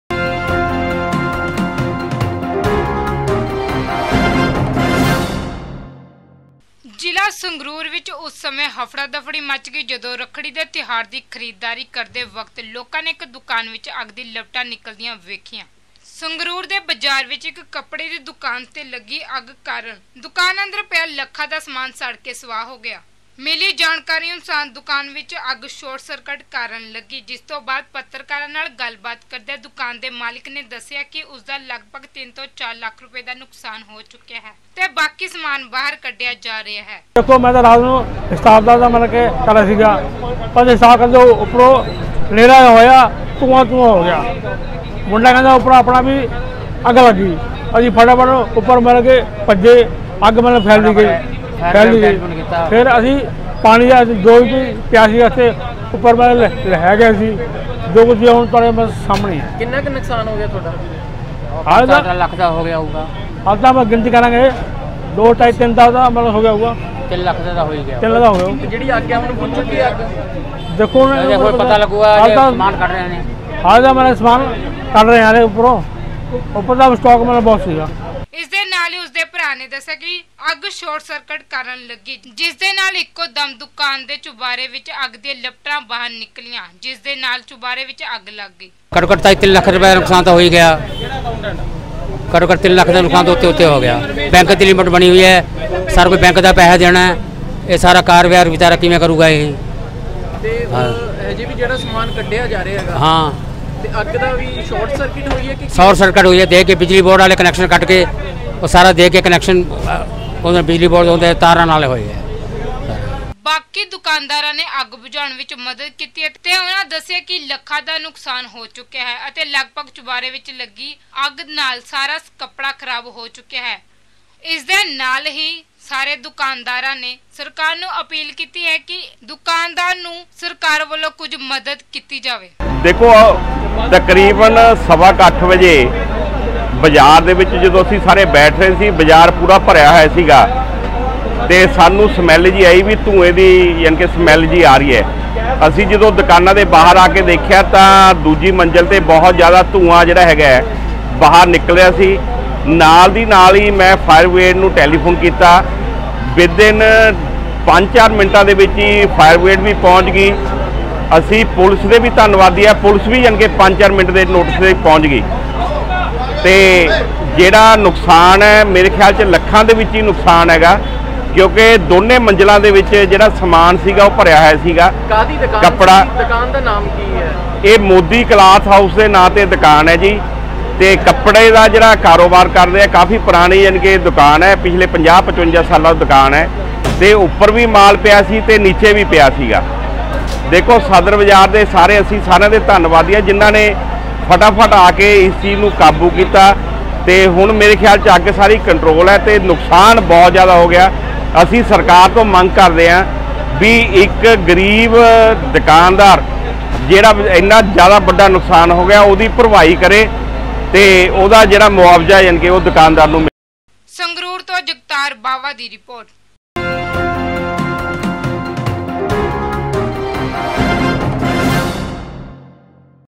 Sangrur | ਸੰਗਰੂਰ ਦੀ ਦੁਕਾਨ ਵਿੱਚ ਲੱਗੀ ਅੱਗ , ਹੋਇਆ ਲੱਖਾਂ ਦਾ ਨੁਕਸਾਨ | Aone News
ਰੱਖੜੀ ਦੇ ਤਿਉਹਾਰ ਬਣਿਆ ਇਸ ਪਰਿਵਾਰ ਲਈ ਮਾਤਮ |